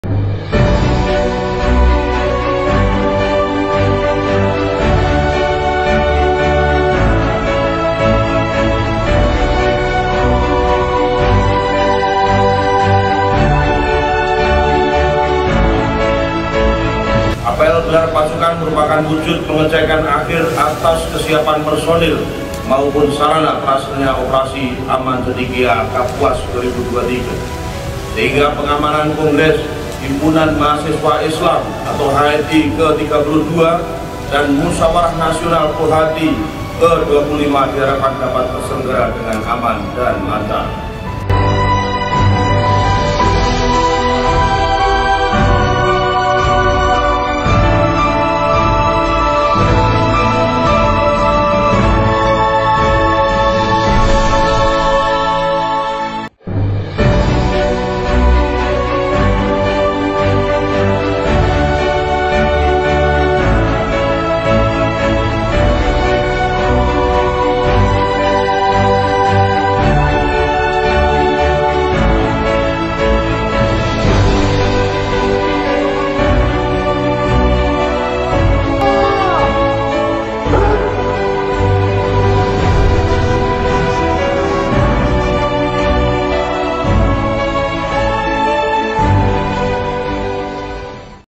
Apel Gelar Pasukan Operasi Aman Cendekia Kapuas 2023
Kapolda Kalbar, Irjen Pol. Pipit Rismanto, S.I.K., M.H. memimpin apel gelar pasukan Operasi Kepolisian “Aman Cendekia Kapuas 2023” di Halaman Jananuraga Mapolda Kalbar, (21/11).

Sebanyak 2.368 personel Polda Kalimantan Barat dikerahkan untuk melaksanakan pengamanan Kongres HMI ke-32 dan Munas Kohati ke-25 di Pontianak pada 24-29 November 2023 yang rencananya akan dihadiri Presiden Joko Widodo.